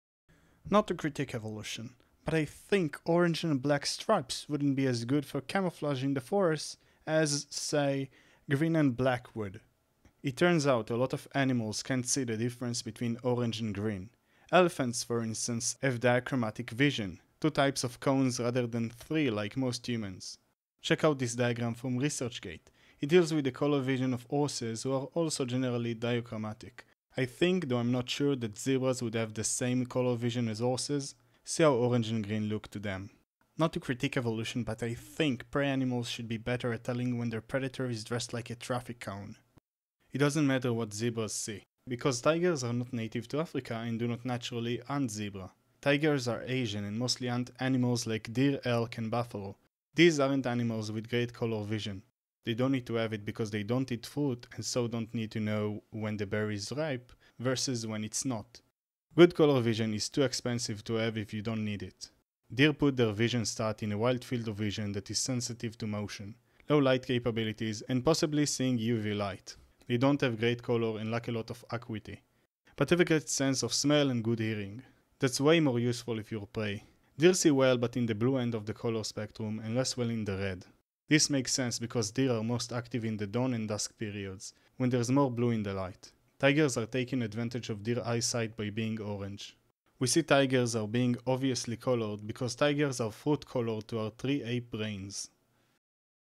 Not to critique evolution
zarekthelordofthefries learns why are tigers orange with the help of acceptableduraz and squeeful, and we get insightful here!!!

Parkour by @koyr1